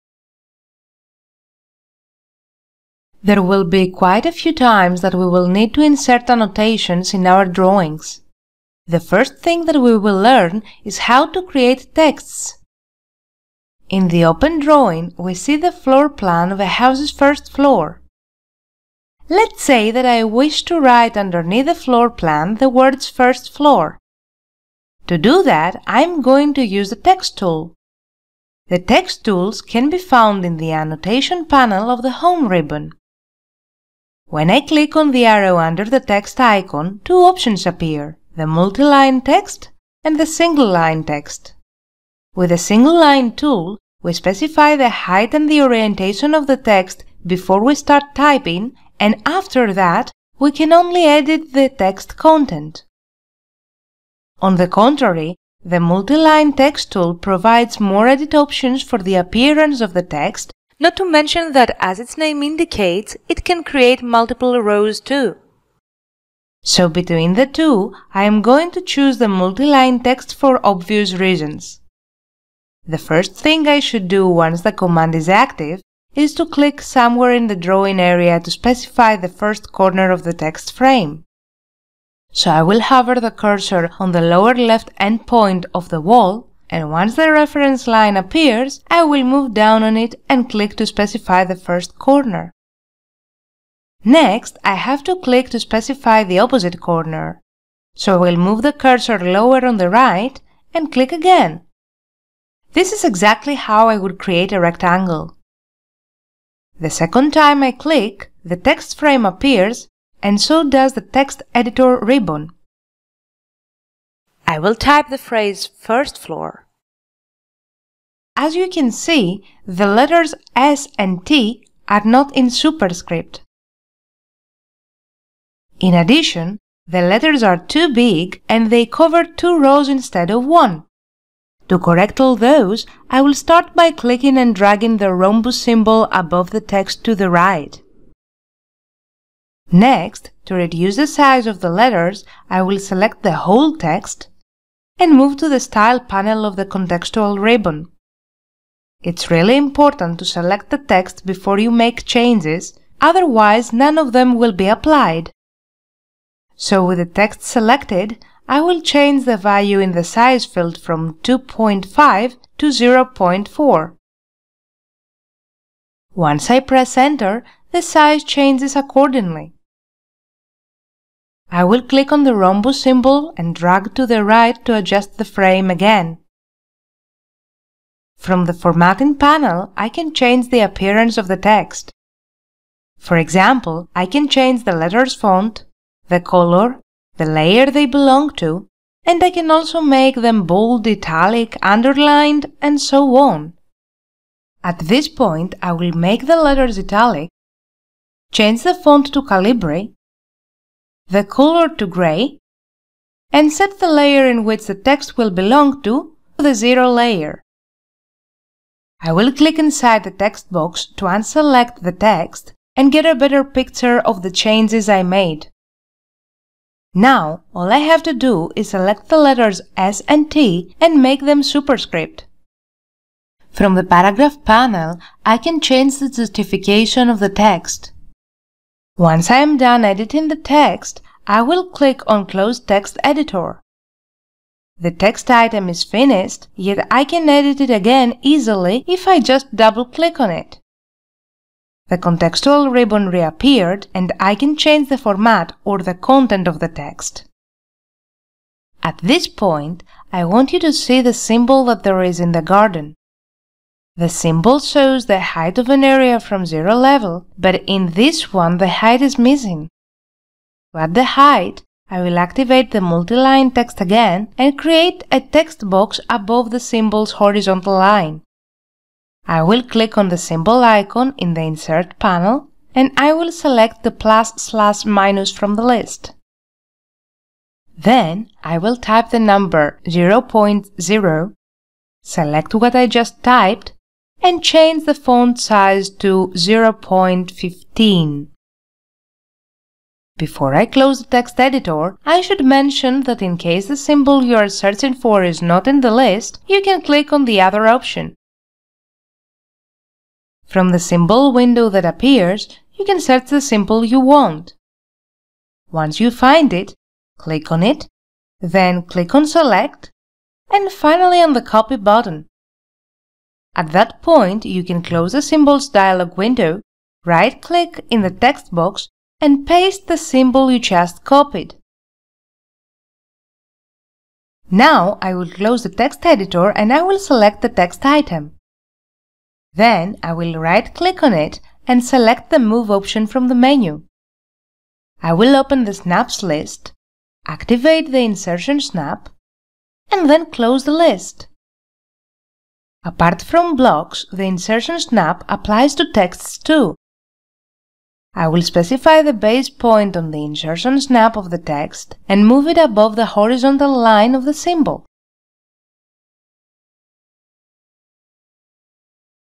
5-2 Creating Texts (AutoCAD 2023)
In this video you will learn how to create text objects. Text objects can include letters, numbers and symbols. You can also modify their font, their size, their color, and the justification of the text.